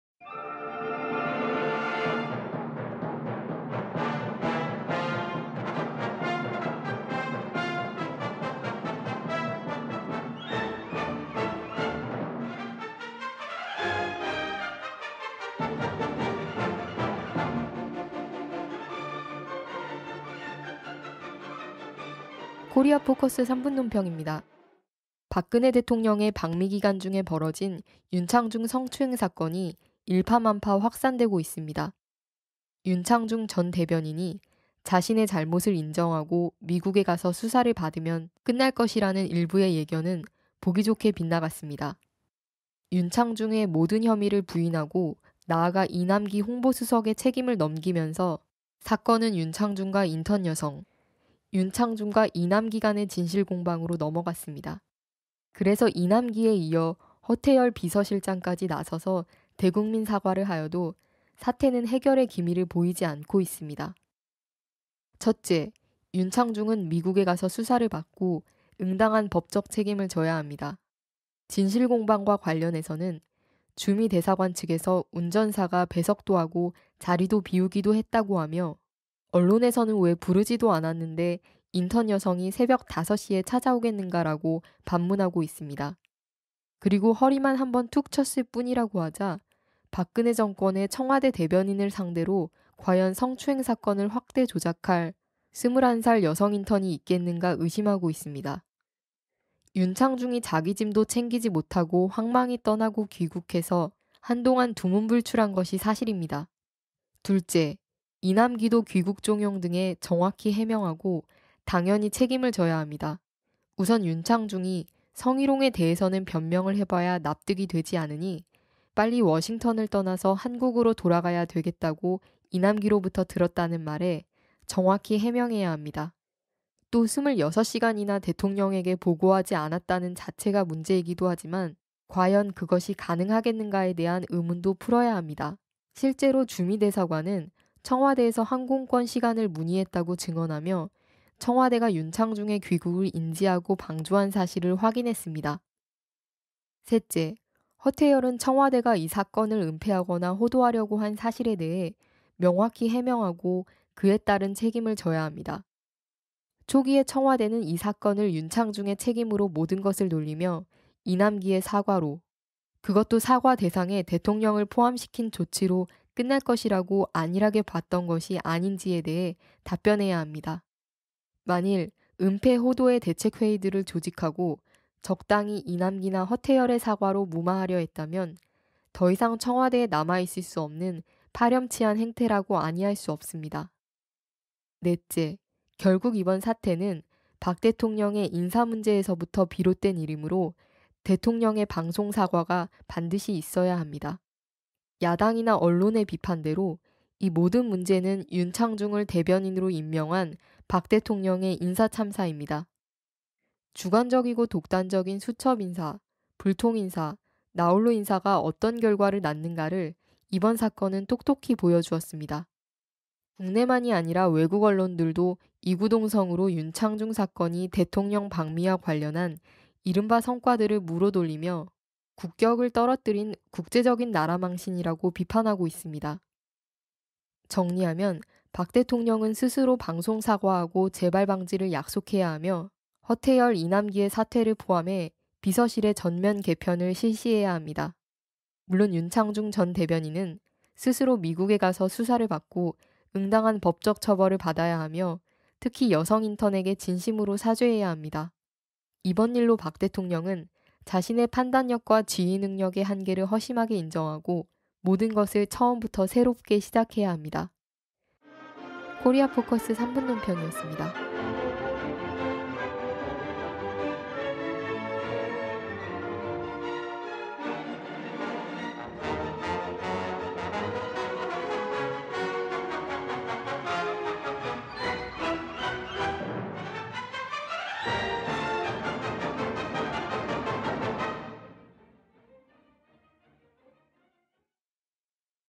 [코리아포커스] 3분논평 : '윤창중'사건은 박대통령의 예견된 인사참사(2013.05.12)
'윤창중'사건은 박대통령의 예견된 인사참사